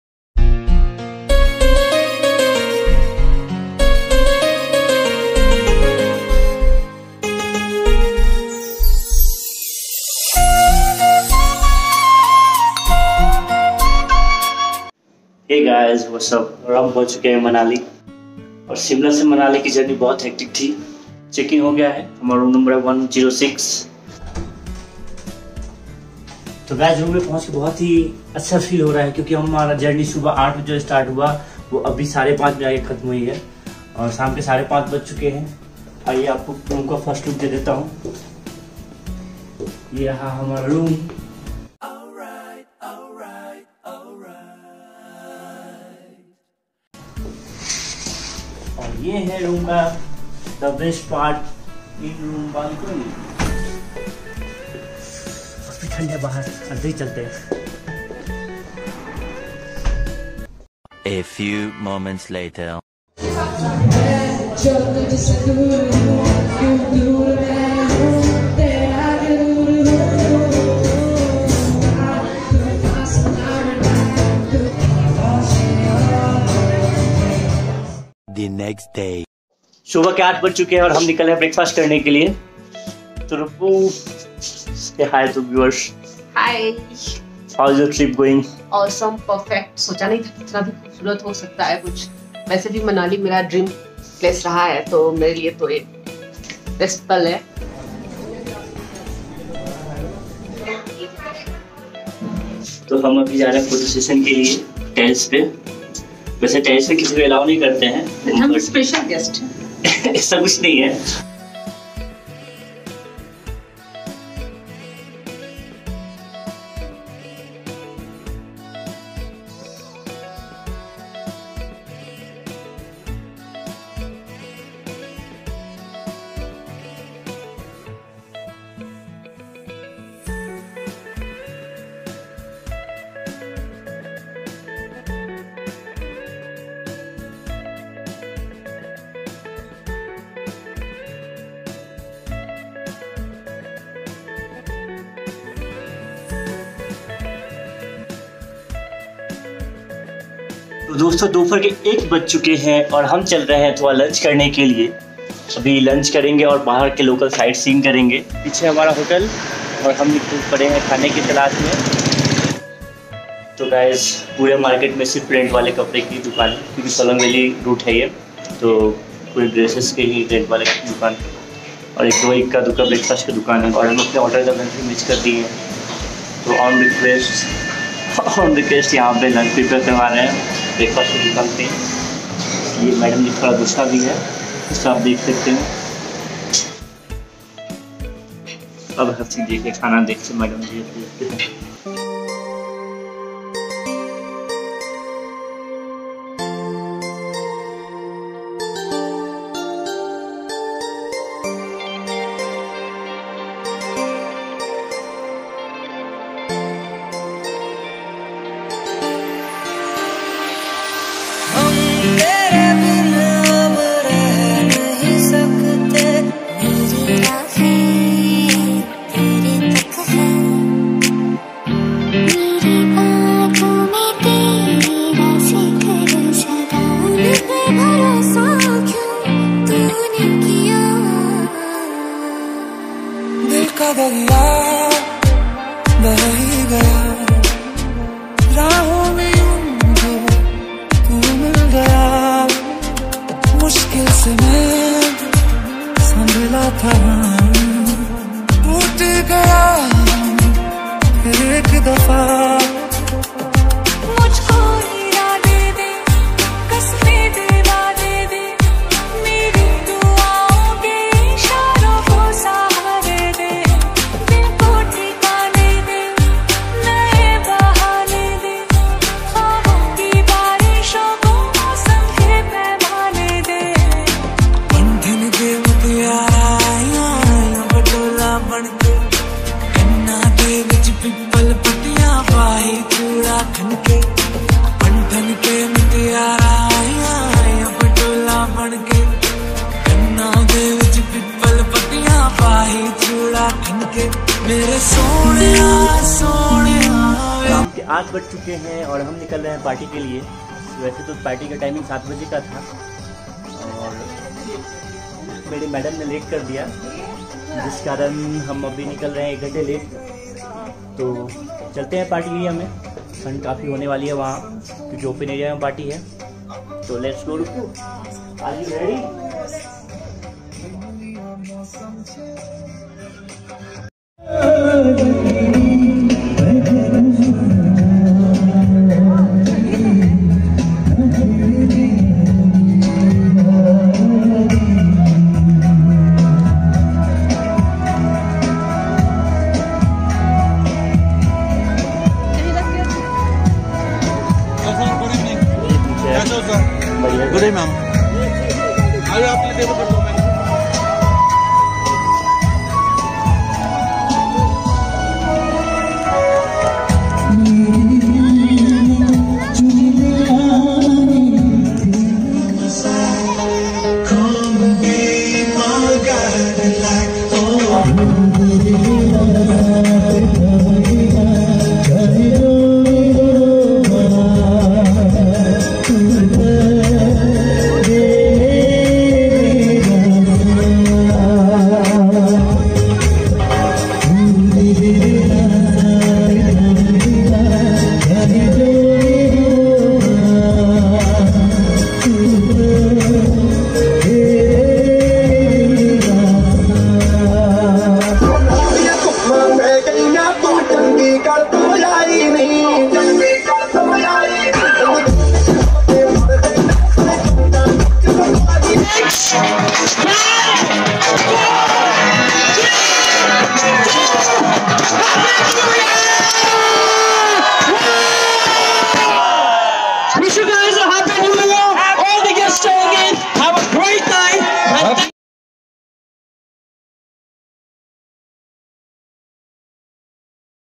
Honeymoon Trip to Shimla-Manali Ep-2
A Romantic trip to Manali
I never knew that a day
Like today lay before us.
I've got the sun in my heart
And my heart's in the sun.…

Honeymoon Trip to Shimla-Manali 2019 EP-2
This time we are in Manali & Enjoyed New year welcome party & many more funs
The allure grand team
Ruppu❤️Raja

5) Twitter @Rise2Ride

6) Hike @rise2ride